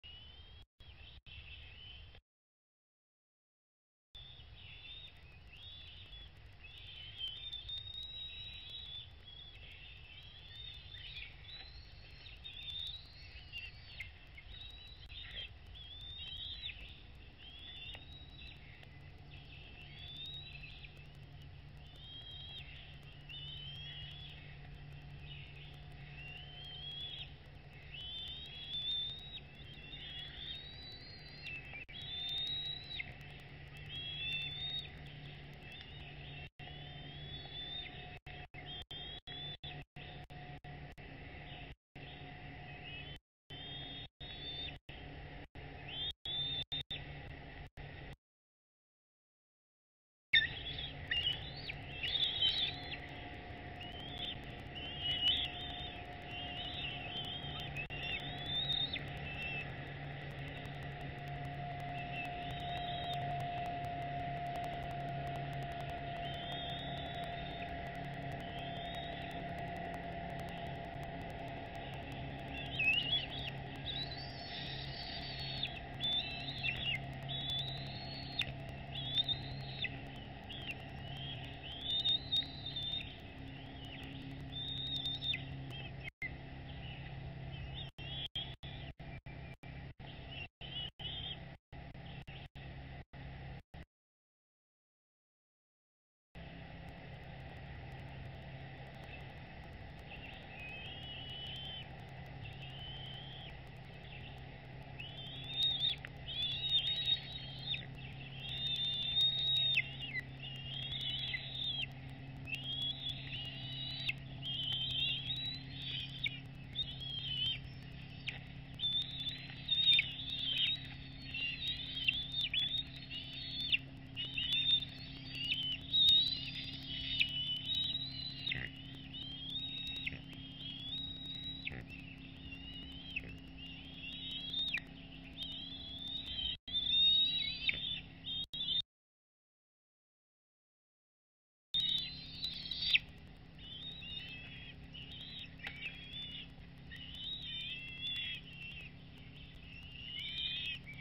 DH2019, DH8 Great Mantle and Fish Fight, 051119
One fish and three hungry eaglets. DH8 does a great mantle and retains the fish when others try to steal it. Lost it momentarily in the fight, but immediately stole it back.